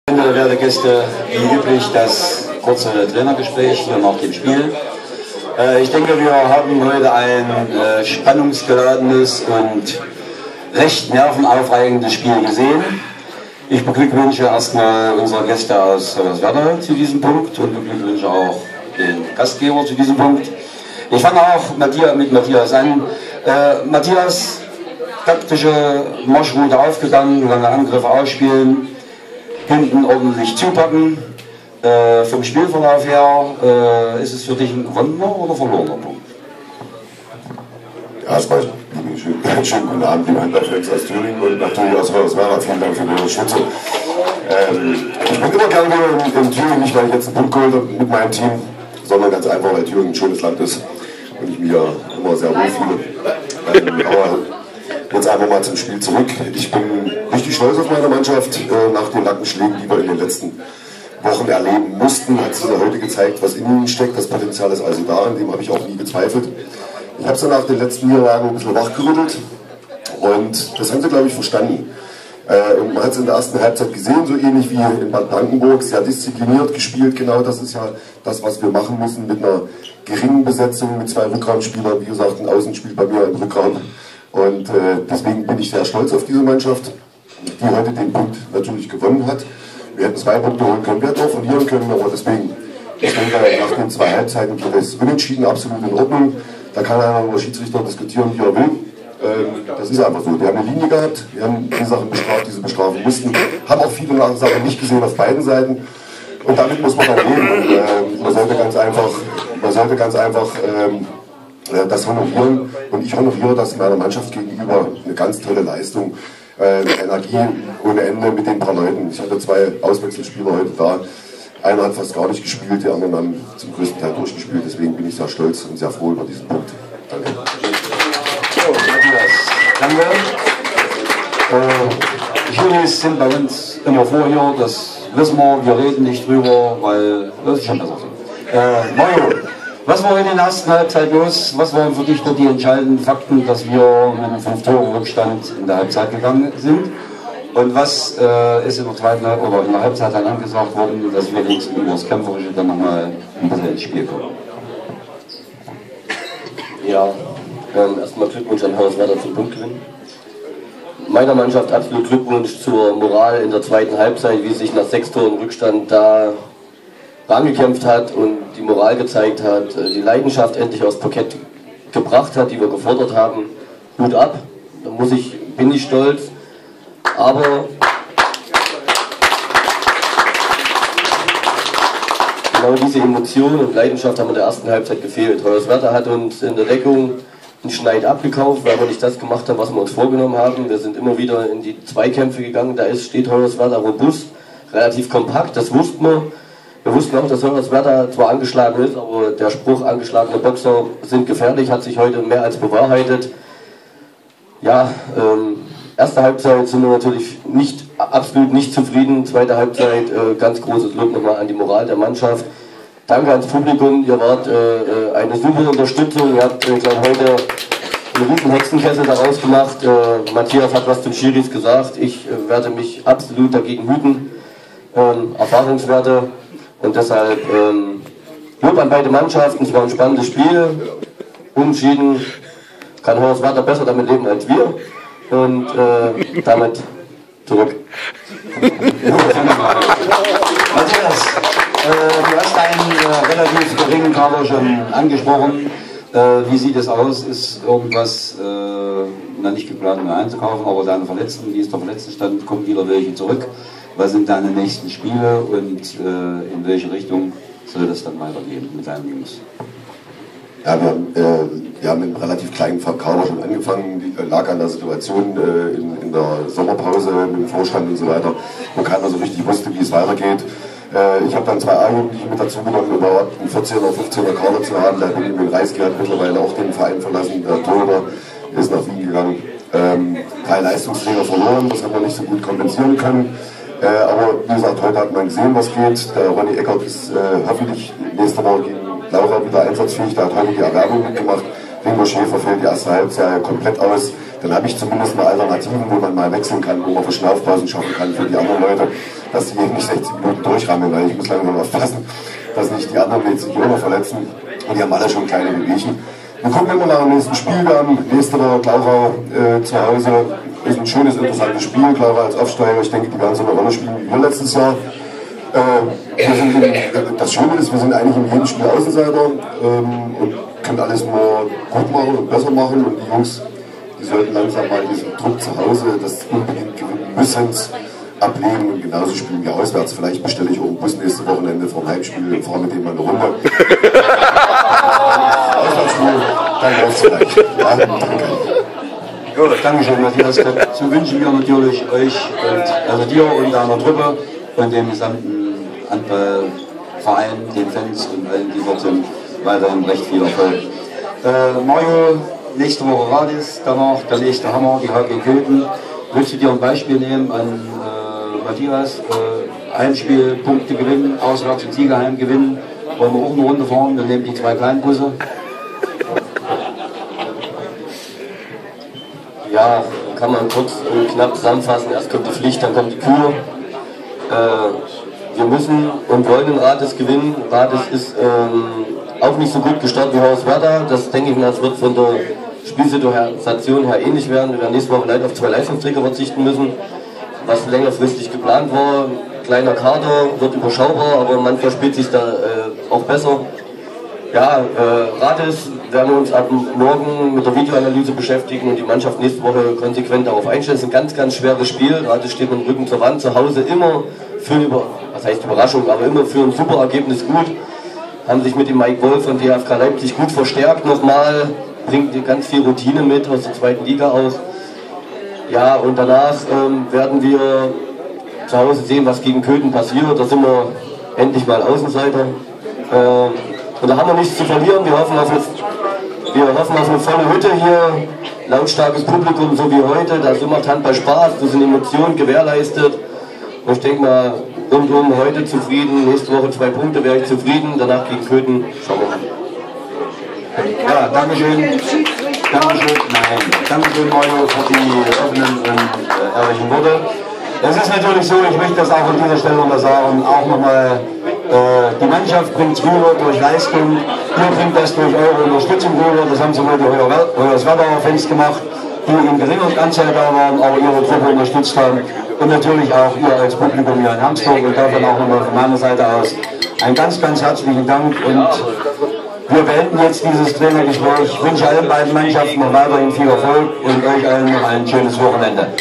Pressekonferenz SV Hermsdorf - LHV Hoyerswerda
Video von der Pressekonferenz vom _Handballspiel des SV Hermsdorf gegen den LHV Hoyerswerda in der Mitteldeutschen Oberliga am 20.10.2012. Das Spiel endete 24:24 Unentschieden, nachdem Hoyerswerda in der Halbzeit mit 10:15 führte